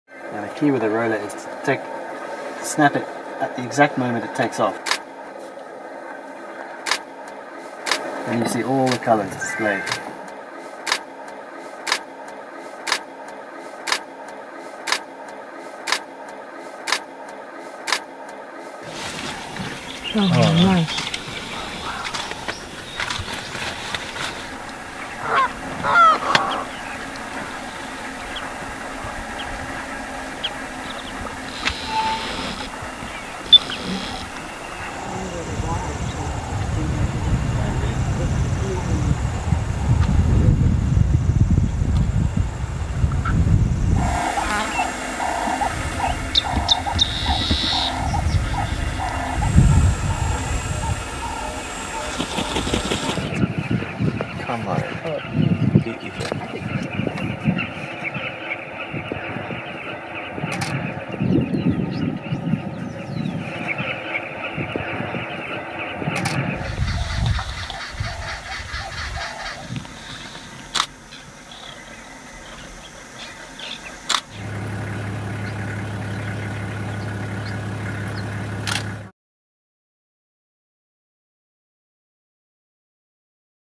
Birds of Botswana
I'm not much of a birdwatcher, but when I was in Botswana I was amazed at the variety of birds.  They came in all shapes and sizes, and some had distinctive features or stunning plumage.  So here are just a few of the birds I saw while on tour with Harkness Safaris, including the lilac-breasted roller, the saddle-billed stork, the African spoonbill, the African darter, the Carmine bee-eater, and the African fish eagle.  Images and video were taken in the Okavango Delta and Chobe National Park in beautiful Botswana.